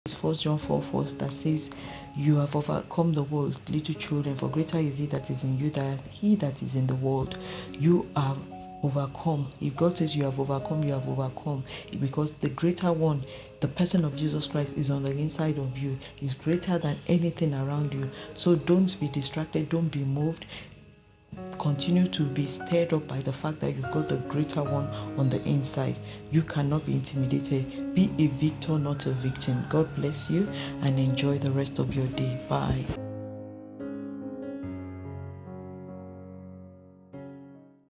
1 John 4 : 4 | Daily Motivation | Pastor Bim
Watch this insightful motivation teaching by Pastor Bim

1 JOHN 4 vs 4 "THAT SAYS YOU HAVE OVERCOME THE WORLD LITTLE CHILDREN FOR GREATER IS HE THAT IS IN YOU, THAN HE THAT IS IN THE WORLD".

1 JOHN 4 vs 4
Ye are of God, little children, and have overcome them: because greater is he that is in you, than he that is in the world. (KJV).

YOU HAVE OVERCOME, IF GOD SAYS YOU HAVE OVERCOME, BECAUSE THE GREATER ONE, THE PERSON OF JESUS CHRIST IS ON THE  INSIDE OF YOU.

HIS GREATER THAN ANYTHING AROUND YOU.  SO DON'T BE DISTRACTED, DON'T BE MOVED, CONTINUE TO BE STIRRED UP BY THE FACT THAT YOU GOT THE GREATER ONE ON THE  INSIDE.

YOU CANNOT BE INTIMATED,
BE A VICTOR AND NOT A VICTIM.

GOD BLESS YOU AND ENJOY THE REST OF YOUR DAY.

 BYE .